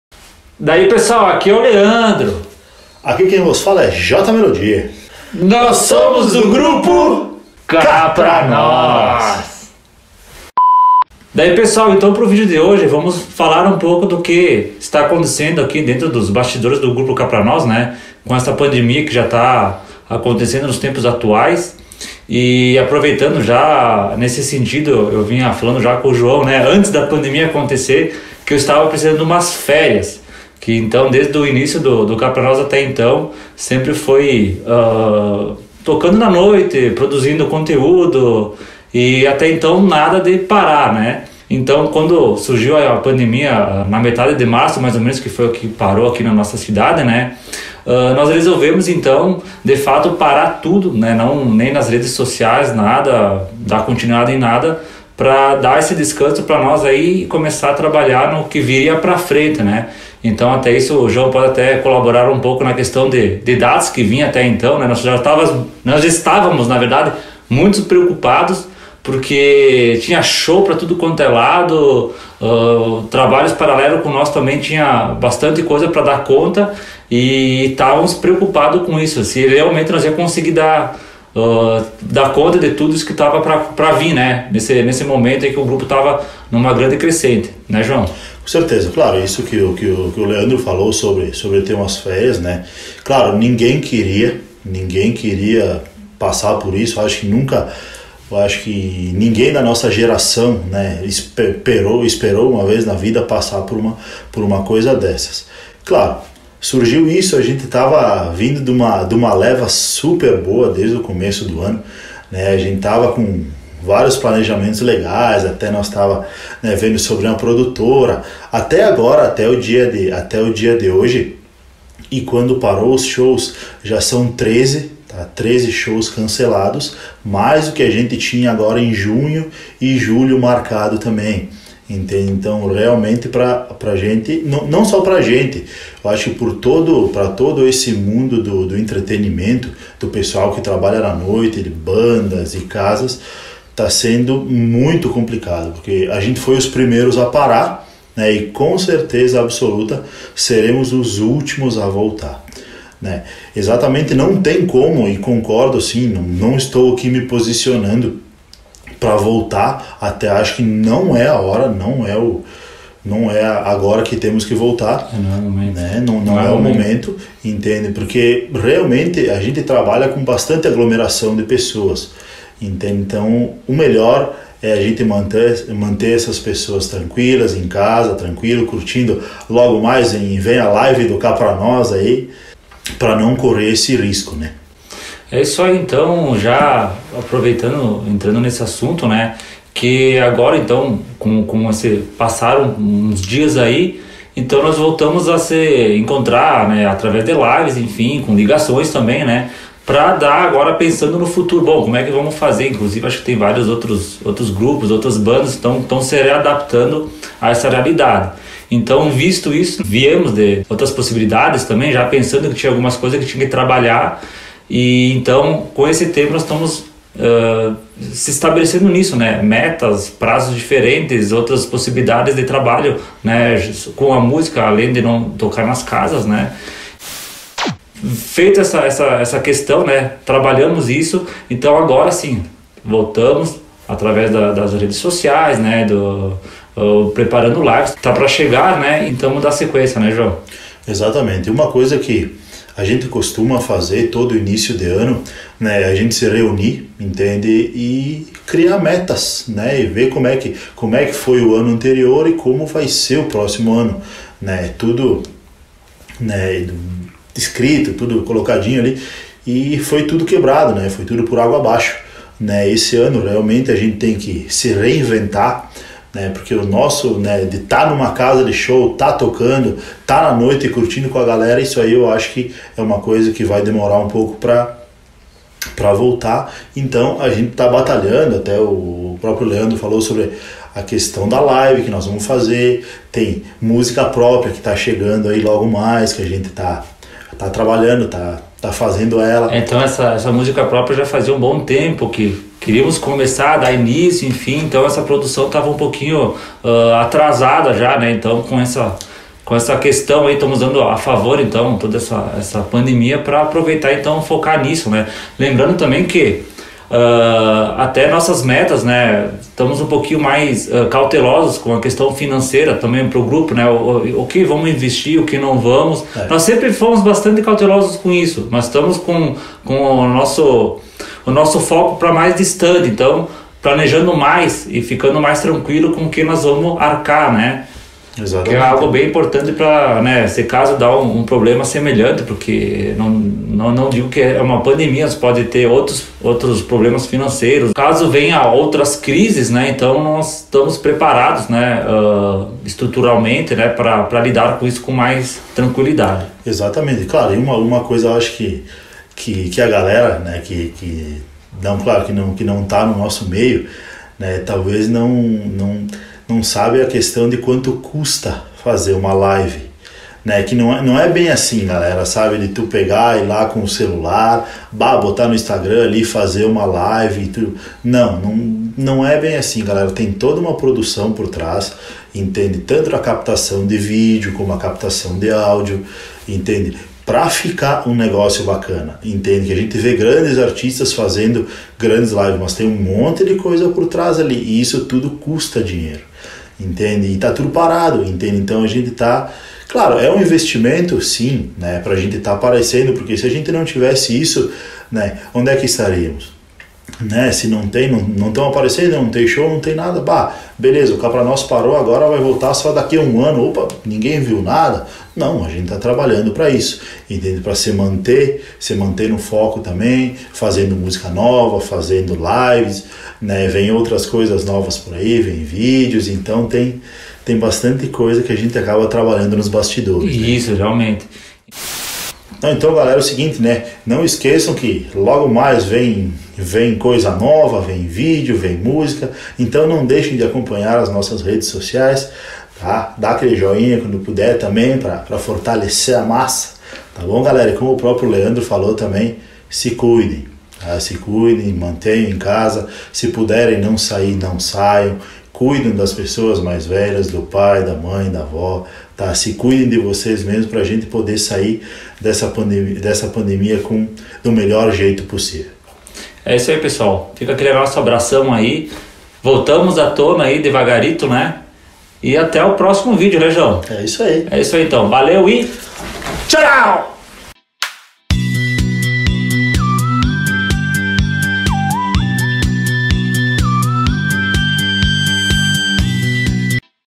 Covid-19
Breve relato do que se passa pelos bastidores do Grupo KapraNós nesse momento delicado de pandemia e perspectivas para o futuro.